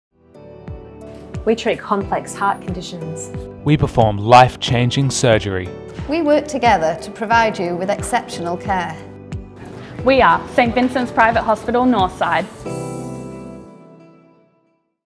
Cardiac Care at St Vincent’s Private Hospital Northside
We treat complex heart conditions. We perform life-changing surgery. We work together to provide you with exceptional care. to discover more about cardiac care at St Vincent's Private Hospital Northside.